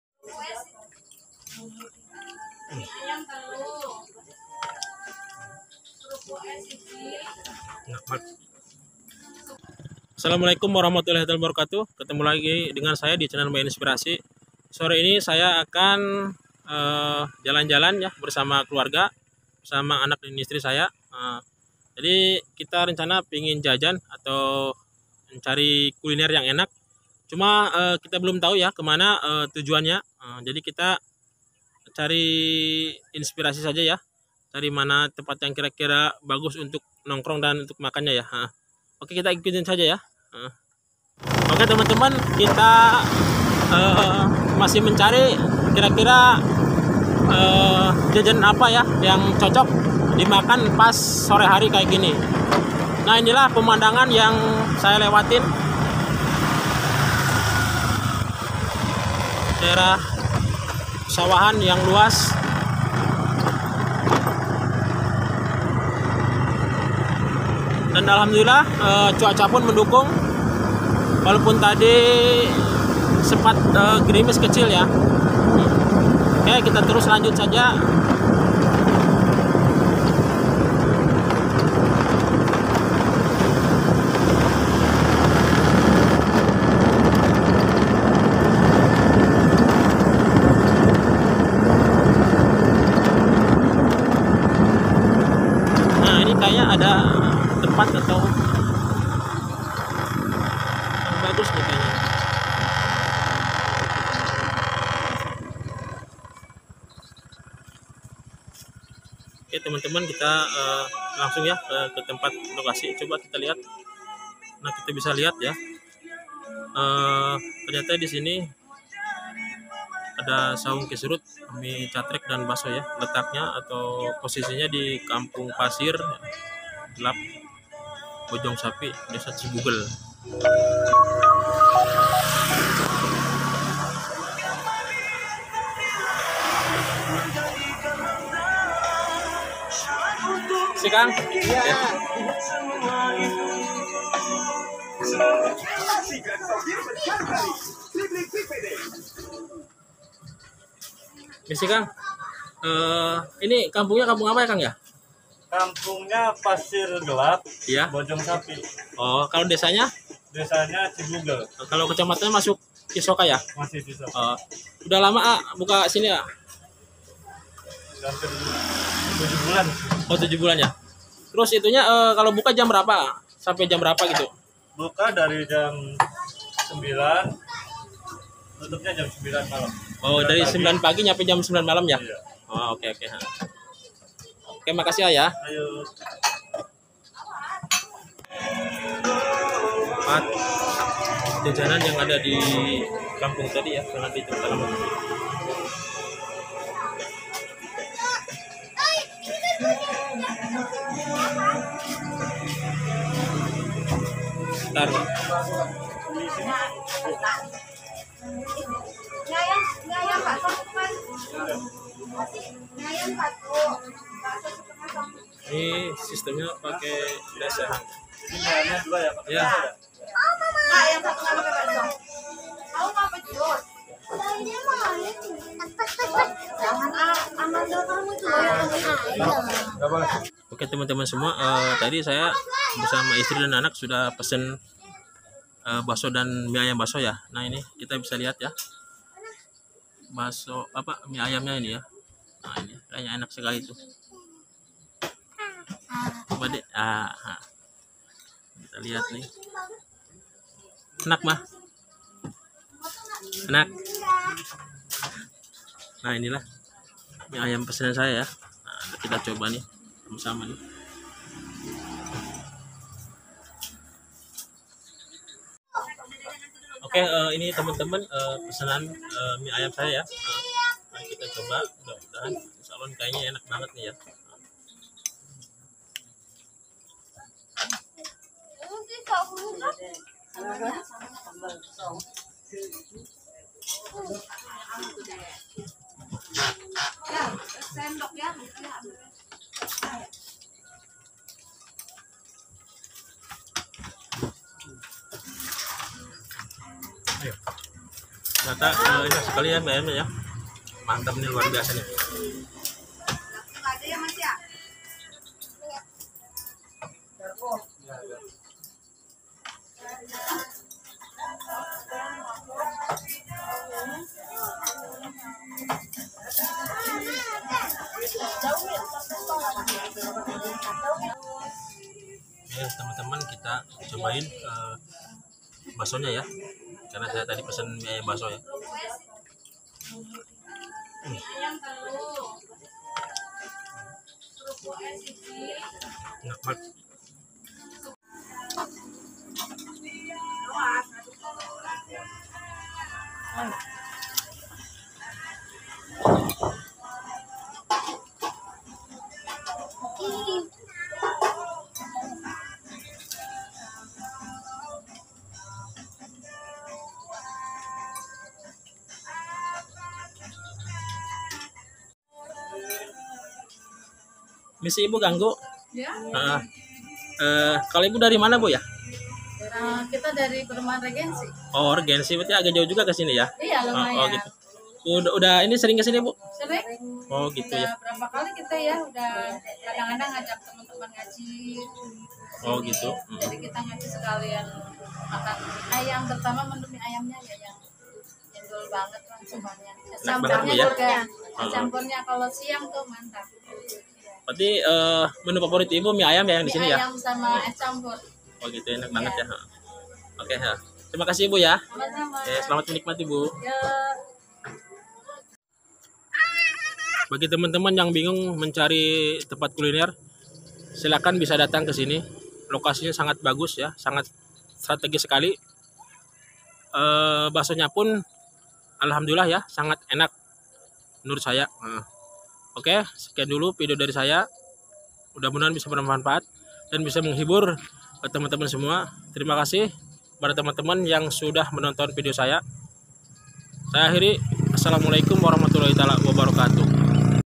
BAKSO DAN MIE AYAM KI SERUT#makanbakso#kulinerjalanan#cisokakuliner
bakso Ki serut terletak di desa ci Bugel kec ci soka Tangerang Banten..merupakan kuliner yang enak tiada duanya..dg sistem lesehan